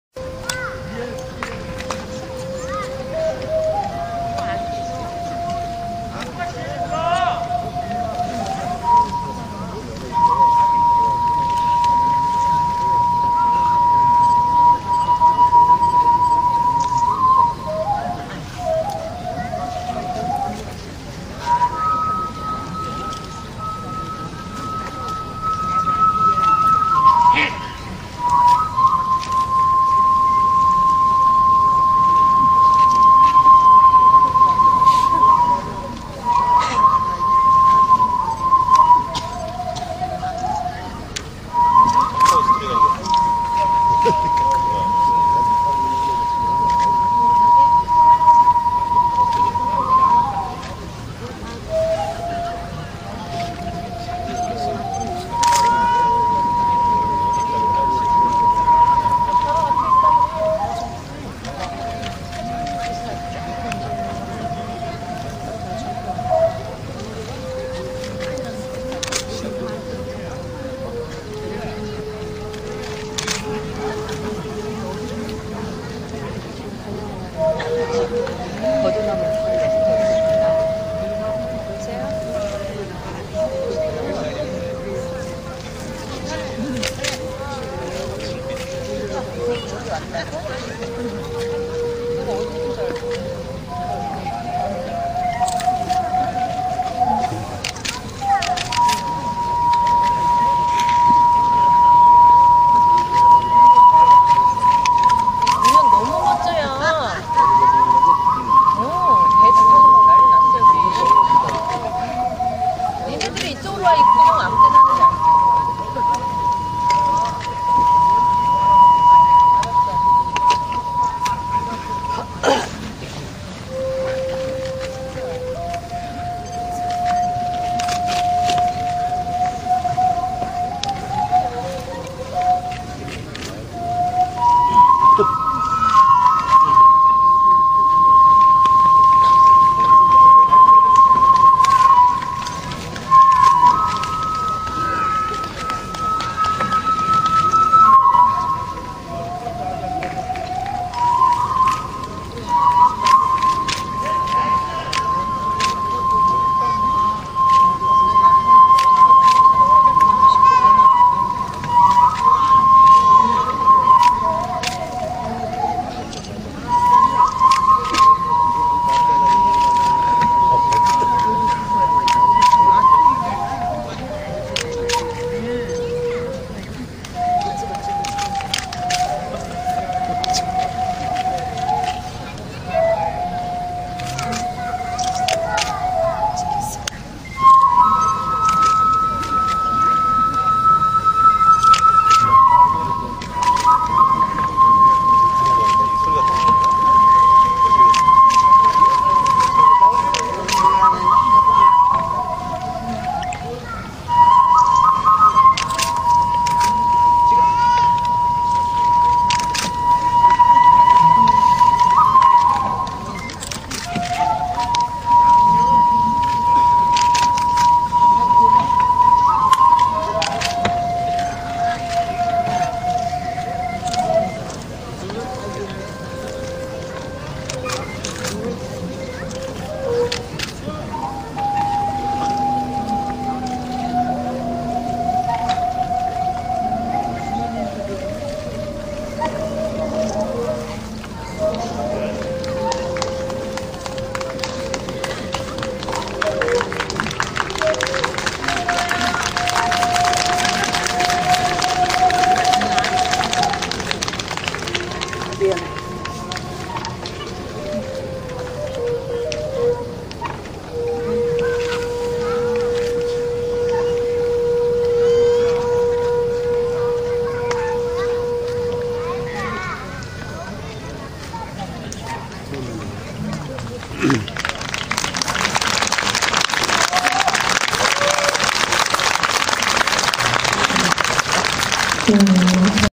龍淵之夢 (Korean folk dance)
Korea(Joseon Dynasty)discrimination,poverty and people 500 years and have a network
of human life in war
Storise were too sad and unfair.
Lull the soul of the dead man unjustly sent to eternal heaven,please
Dance to express appearance.